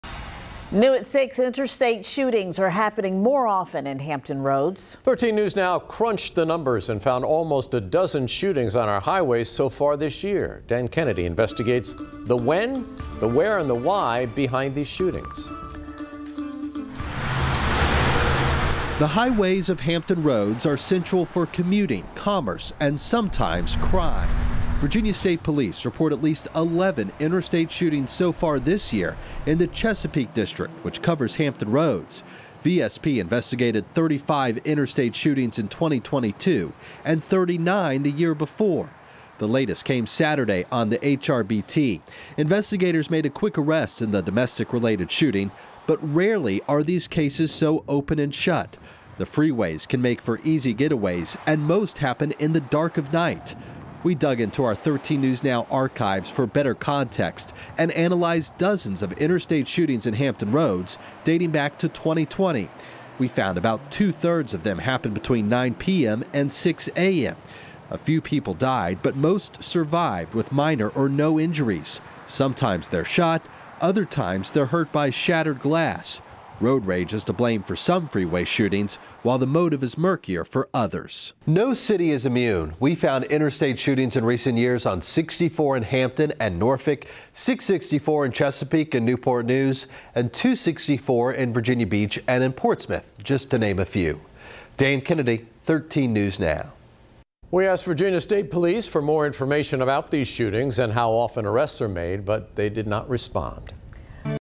Interstate shootings happening more frequently across Hampton Roads
So far, almost a dozen shootings have been reported on the freeways across the region. Dan Kennedy takes a look at the numbers dating back to 2020.